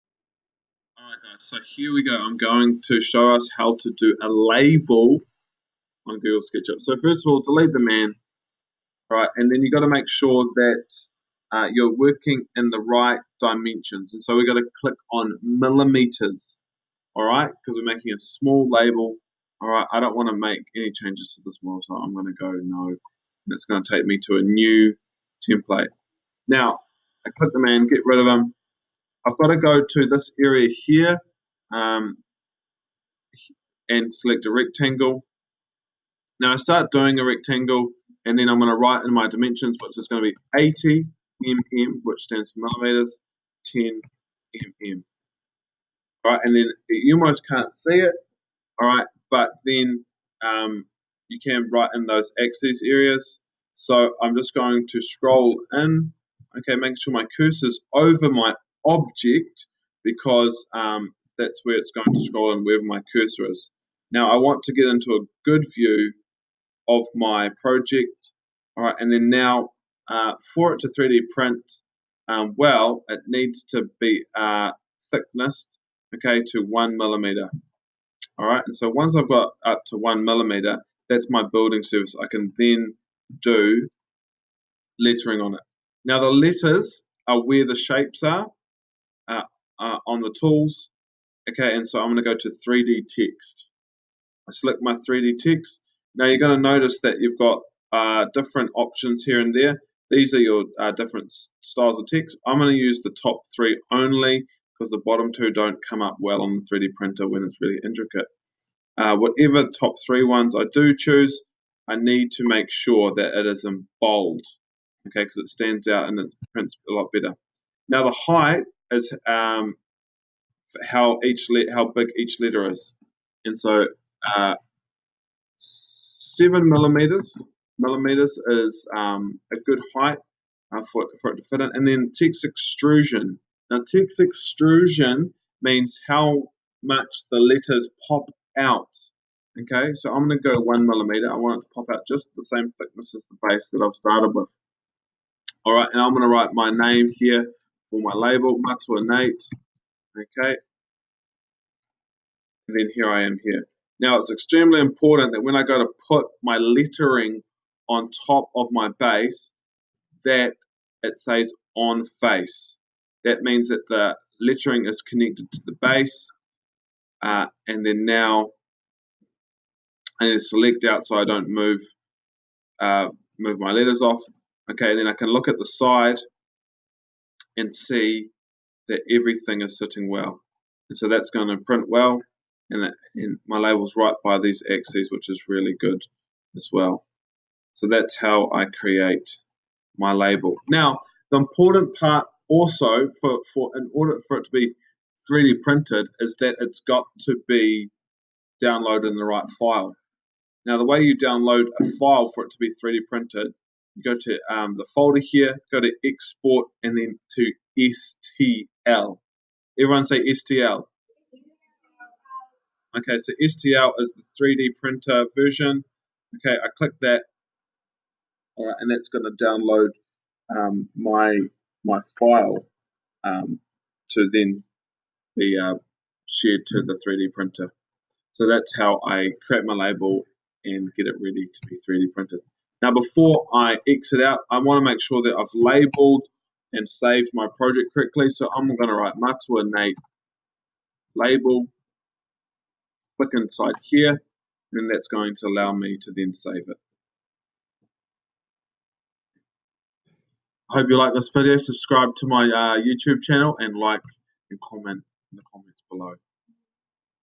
How to Create a Label on Sketchup
In this video you learn how to create a simple label on Sketchup that is 3d printable.